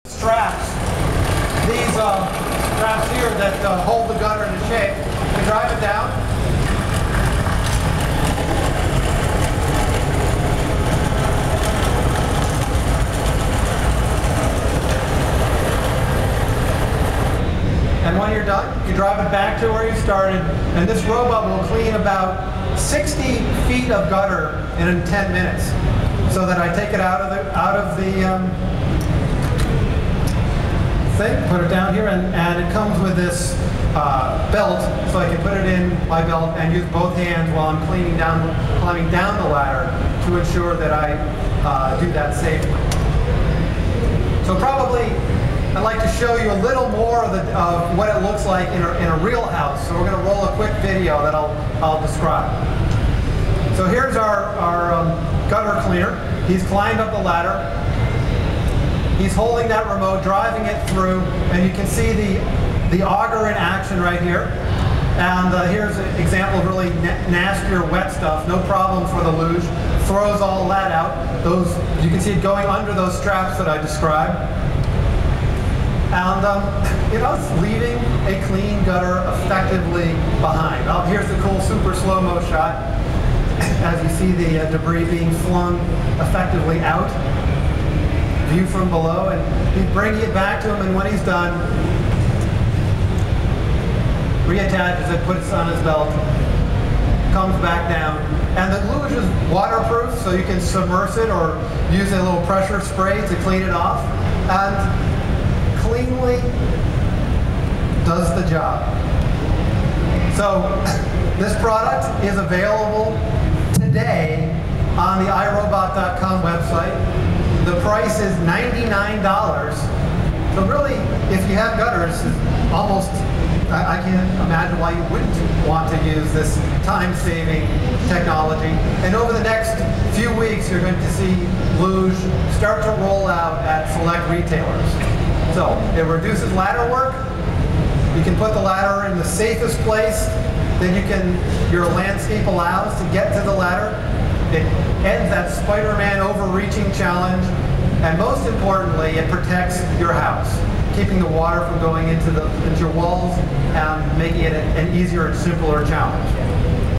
Colin Angle demonstrates the iRobot Looj
Colin angle demonstrates the iRobot Looj Robotic Gutter Cleaner at Digital Life 2007 from RoombaReview.com